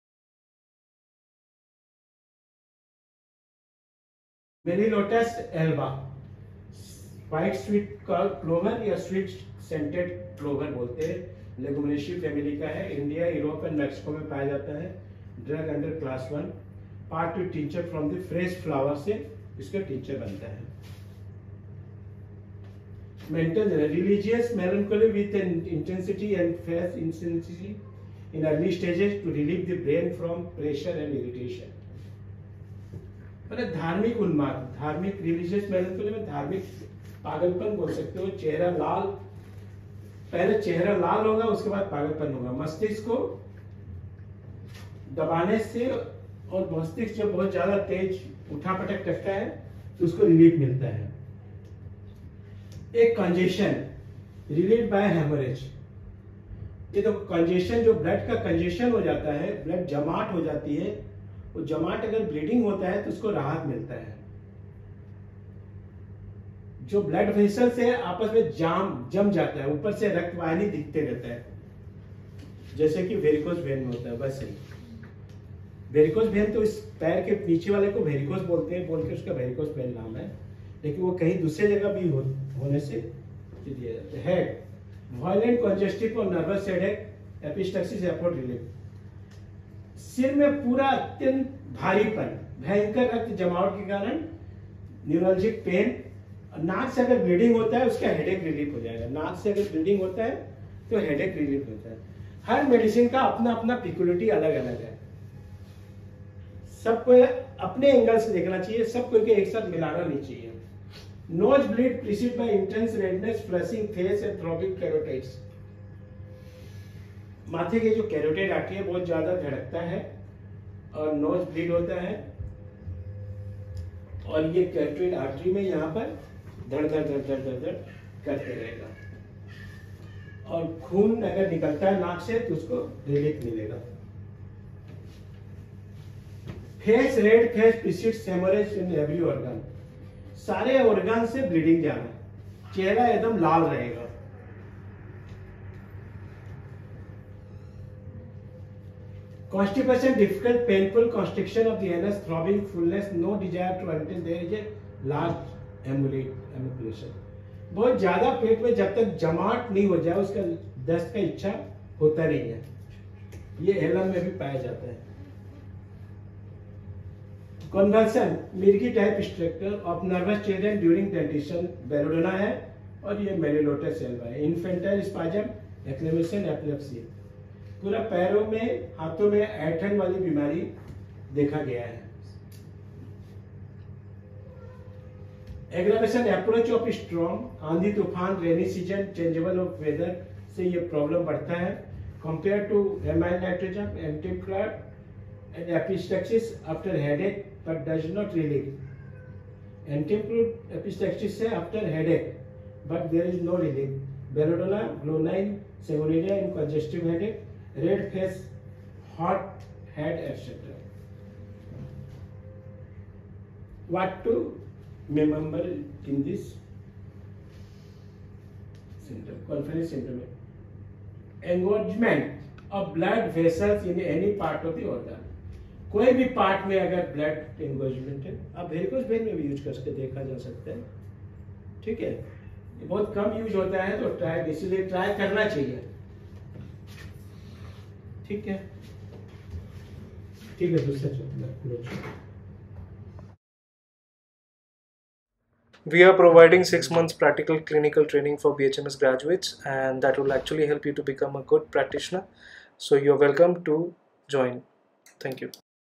Melilotus alba Allen SD 480p
Melilotus Alba.
Sweet Clover -Leguminosae.-
Congestions, relieved by haemorrhage. Engorgement of blood-vessels in any part of organ. Violent congestive or nervous headaches; epistaxis affords relief -Bufo, Ferr. p., Magn. s.-. Convulsions: of nervous children during dentition -Bell.-; infantile spasms, eclampsia, epilepsy. Religious melancholy with an intensely red face; insanity, in early stages, to relieve brain from pressure and irritation. Nosebleed preceded by intense redness, flushing of face and throbbing of carotids -Bell.-; with general relief. Very red face precedes haemorrhage from every organ. Constipation: difficult, painful, constriction in anus; throbbing, fulness; no desire until there is a large accumulation -Alum.-.
Relations. - Compare: Amyl., Ant. c., in epistaxis after headache, but does not relieve; Bell., Glon., Sang. in congestive headache, red face, hot head, etc.
Aggravation. - Approach of a storm; rainy, changeable weather.